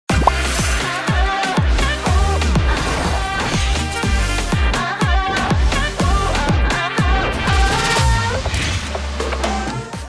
Spray desenderante Amor de Verano 💛
¡En tu bolso de playa, no puede faltar el Spray Desenredante Amarás Amor de Verano💛! Este nuevo must, hidrata tu melena y la protege de los rayos UV. ¡Prueba ya este infaltable de verano, recuerda que, como toda la colección, es edición limitada!​⛱️🌤️💛